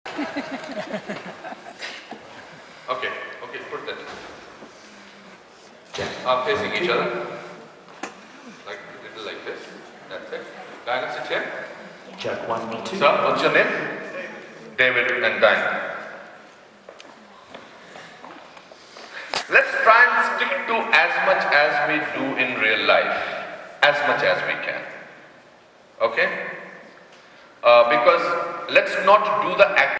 Diana Durango & David McRobert with Bollywood Actor Anupam Kher during IIFA - 2014
Diana Durango & David McRobert shared stage with Bollywood Actor Anupam Kher during IIFA Awards Tampa , Fl - 2014
Camera: Anjani Pandey of Nukkad-USA Productions.